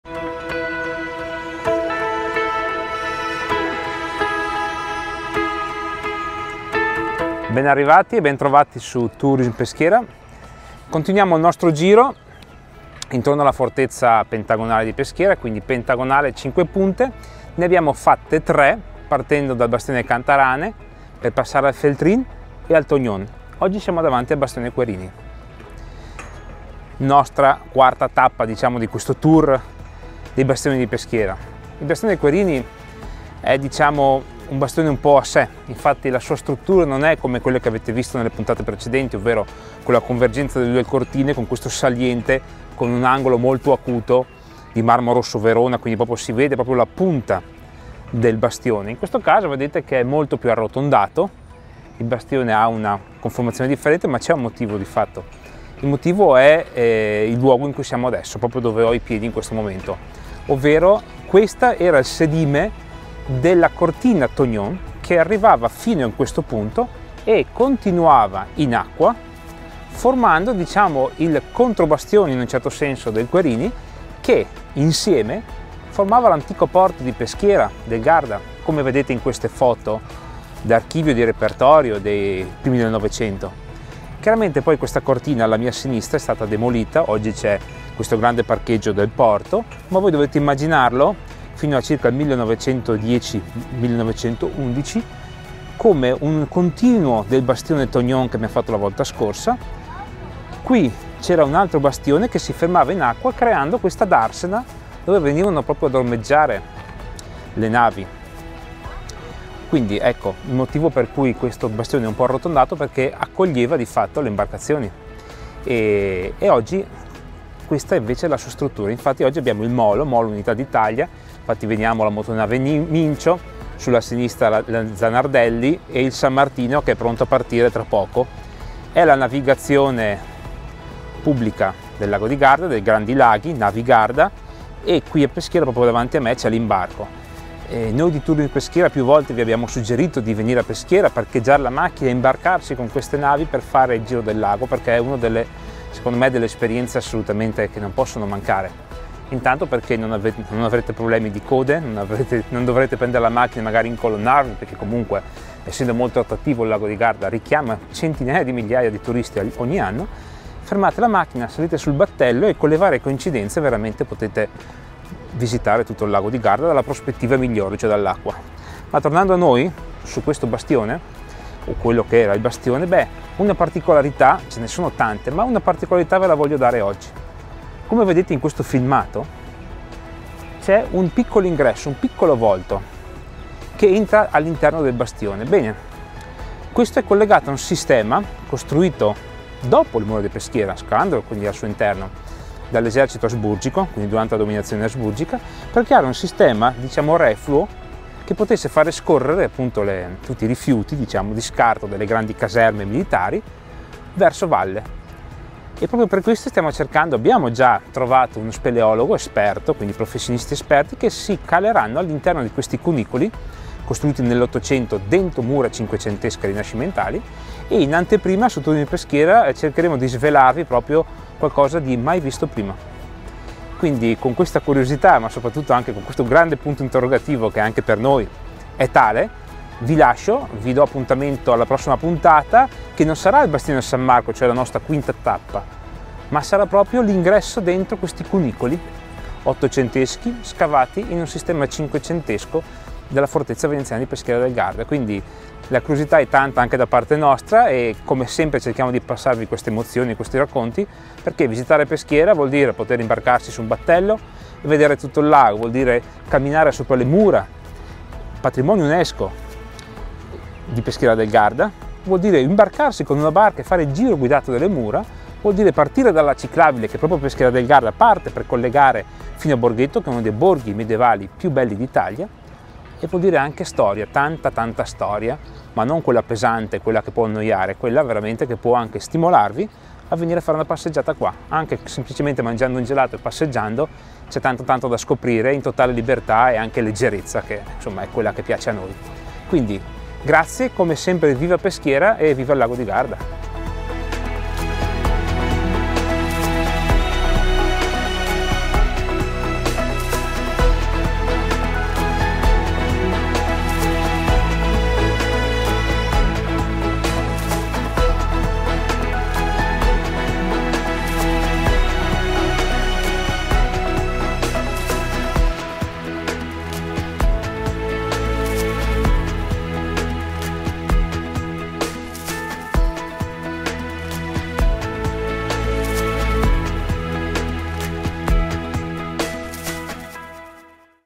Il Bastione Querini - 30 maggio 2022
“Gli approfondimenti culturali del mercoledì di Tourism Peschiera”
Quarta tappa del nostro viaggio alla scoperta dei cinque Bastioni della fortezza di Peschiera del Garda...vi presentiamo oggi, su "The Storytelling Channel", il Bastione Querini, per gli approfondimenti del mercoledì di Tourism Peschiera.